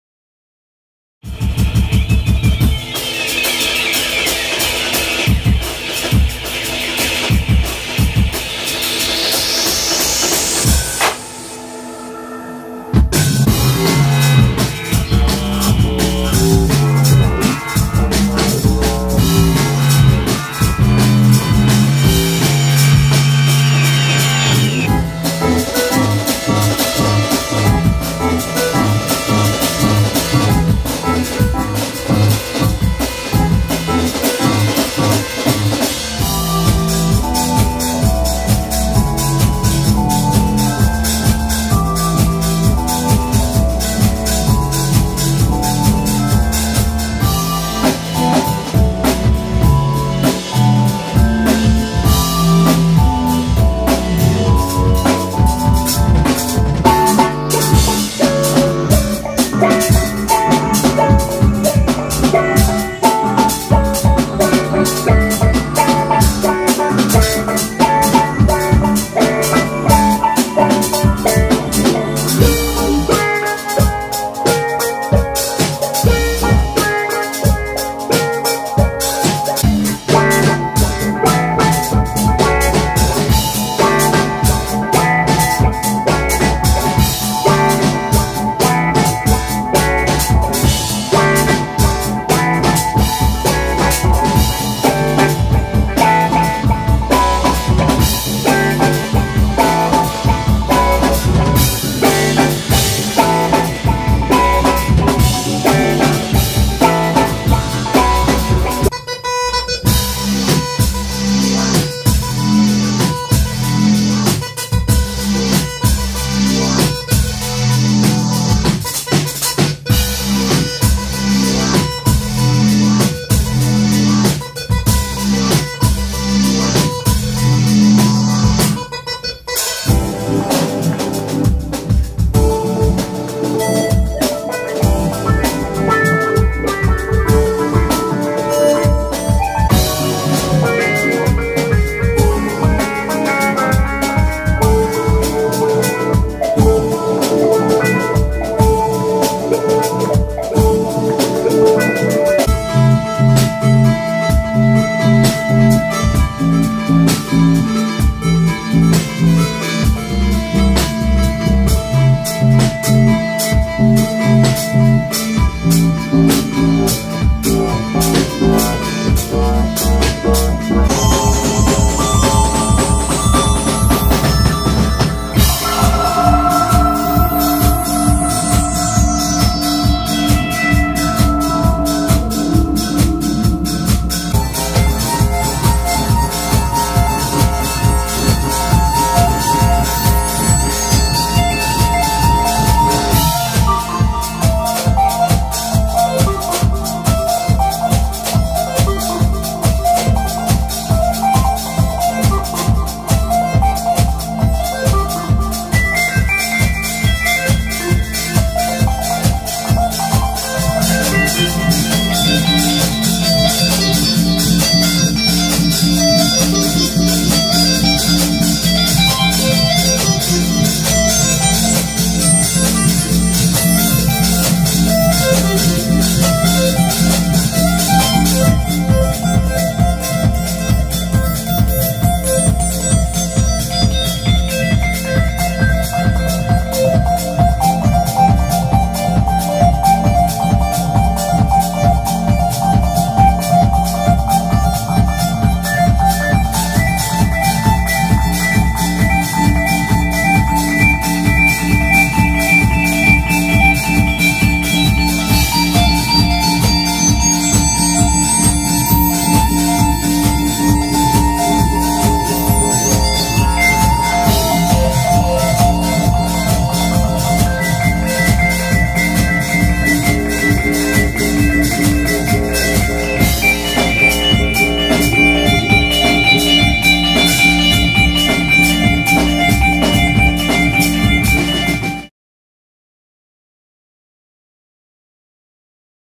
catena 190918 Live at NEPO
catena (カテナ)

key 河原崎豊 Yutaka Kawarasaki
bass 位髙太一 Taichi Yagoto
 ds  吉原 誠   Makoto Yoshihara